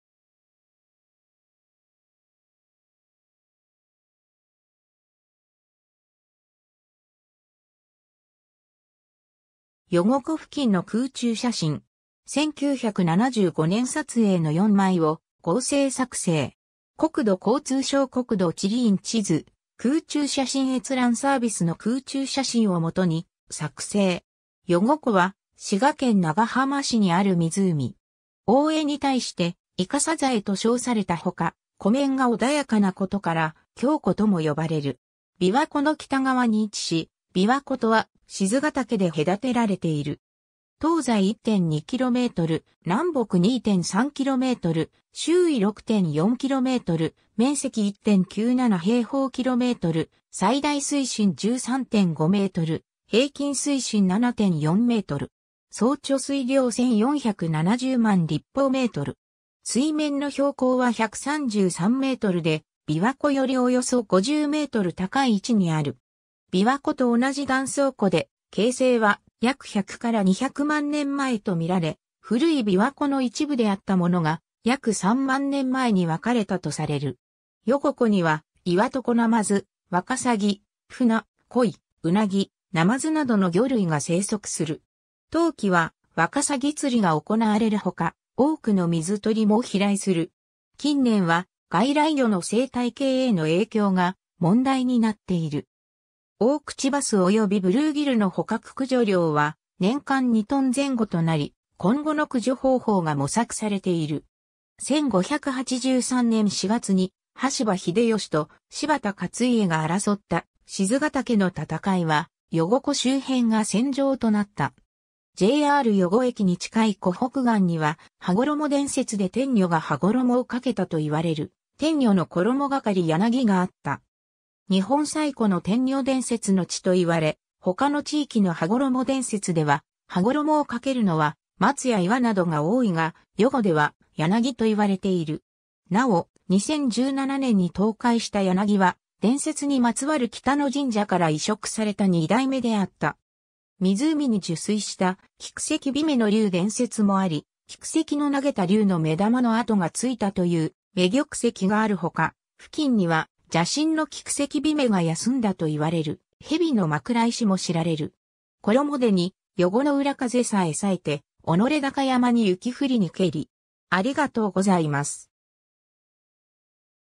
余呉湖
余呉湖付近の空中写真。
1975年撮影の4枚を合成作成。
 余呉湖（よごこ、よごのうみ）は、滋賀県長浜市にある湖。
「大江」（琵琶湖）に対して「伊香小江（いかごのおえ）」と称されたほか、湖面が穏やかなことから「鏡湖」とも呼ばれる。
 琵琶湖の北側に位置し、琵琶湖とは賤ヶ岳（標高422m）で隔てられている。
東西1.2キロメートル (0.9km)、南北2.3キロメートル (1.8km)、周囲6.4キロメートル（6.2km）、面積1.97平方キロメートル、最大水深13.5メートル（14.5m、13.0m）、平均水深7.4メートル。
総貯水量1470万立方メートル。
水面の標高は133メートル (132.8m) で、琵琶湖よりおよそ50メートル（約47.0m）高い位置にある。
琵琶湖と同じ断層湖で、形成は約100-200万年前と見られ、古い琵琶湖の一部であったものが約3万年前に分かれたとされる。
 余呉湖にはイワトコナマズ（余呉湖・琵琶湖にのみ生息）、ワカサギ、フナ、コイ、ウナギ、ナマズなどの魚類が生息する。
冬季はワカサギ釣りが行われるほか、多くの水鳥も飛来する。
近年は外来魚の生態系への影響が問題になっている。
オオクチバスおよびブルーギルの捕獲駆除量は年間2トン前後となり、今後の駆除方法が模索されている。
 1583年4月に羽柴秀吉と柴田勝家が争った賤ヶ岳の戦いは余呉湖周辺が戦場となった。
 JR余呉駅に近い湖北岸には、羽衣伝説で天女が羽衣を掛けたといわれる「天女の衣掛柳」があった（2017年〈平成29年〉10月、台風により倒壊）。
日本最古の天女伝説の地といわれ、ほかの地域の羽衣伝説では、羽衣を掛けるのは松や岩などが多いが、余呉では柳といわれている。
なお、2017年（平成29年）に倒壊した柳（マルバヤナギ）は、伝説にまつわる北野神社から移植された2代目であった。
 湖に入水した菊石姫の龍伝説もあり、菊石の投げた龍の目玉の跡がついたという「目玉石」があるほか、付近には蛇身の菊石姫が休んだといわれる「蛇の枕石」も知られる。
 衣手に余呉の浦風さえさえて己高山（こだかみやま）に雪降りにけり （源頼綱 「近江百人一首」『金葉和歌集』）